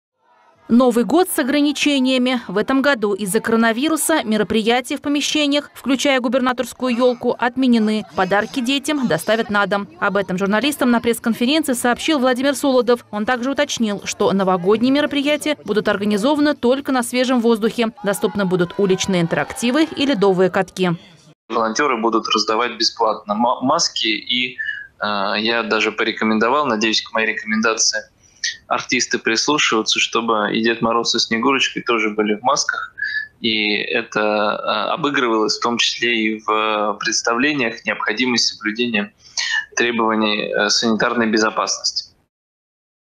Новый год с ограничениями
В этом году из-за коронавируса мероприятия в помещениях, включая губернаторскую елку отменены. Подарки детям доставят на дом.Об этом журналистам на пресс-конференции сообщил Владимир Солодов. Он также уточнил, что новогодние мероприятия будут организованы только на свежем воздухе. Доступны будут уличные интерактивы и ледовые катки.